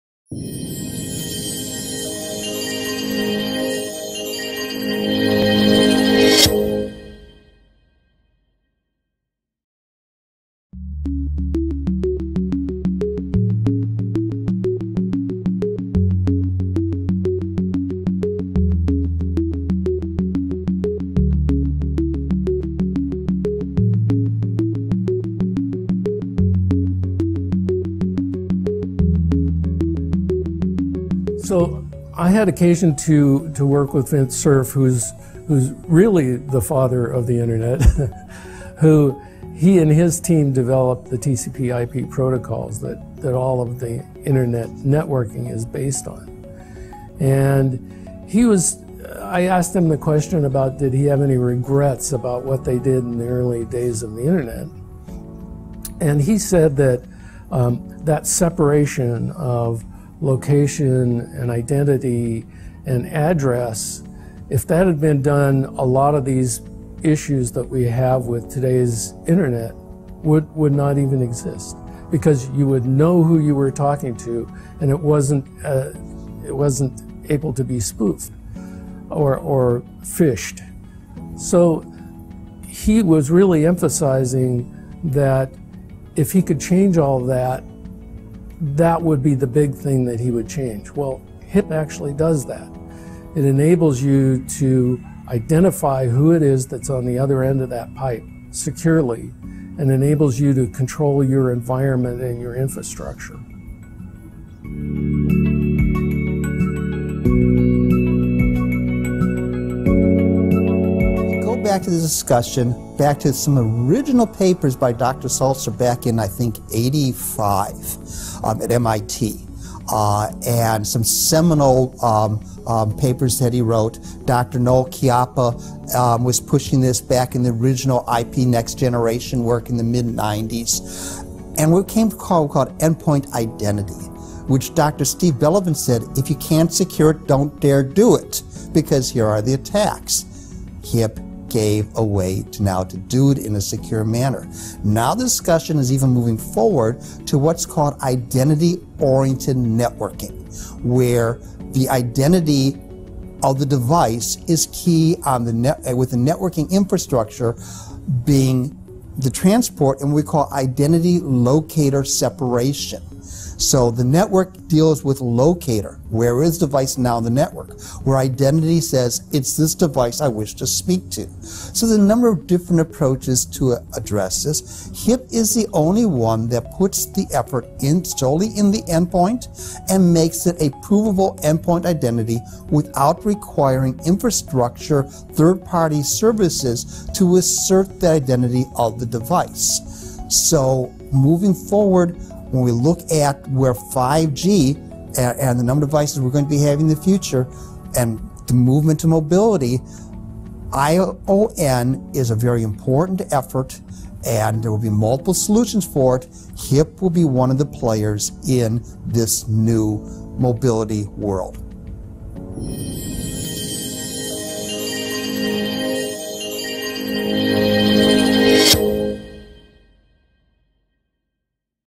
The Host Identity Protocol
The Host Identity Protocol is an open standard that fixes the broken trust model of current IP networking, by introducing cryptographic identities and a new secure namespace. HIP was first proposed by Bob Moskowitz in 1999 as a way to solve the fundamental flaw in TCP/IP networking: using an IP address for both location and identity of a host or service on the network.

Ratified by the Internet Engineering Task Force (IETF), HIP delivers an improved IP architecture with authentication, authorization, and IP mobility based on cryptographic identities.